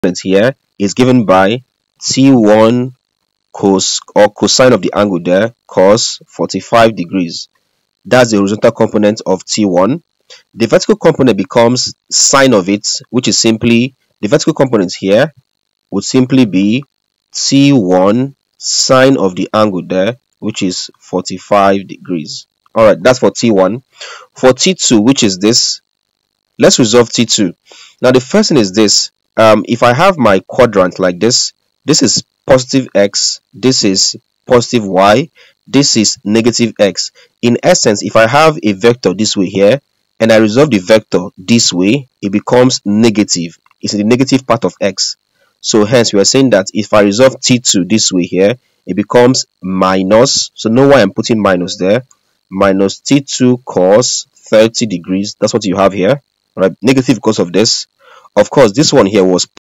Resolution of Vectors | Physics Mechanics: Application to Finding the Tension in a String.
In this Physics tutorial video, I explain the resolution of vector and how to apply the resolution of vectors to finding the tension in a string. Watch this video to learn about the resolution of vecrors and it's application to finding tensions in strings.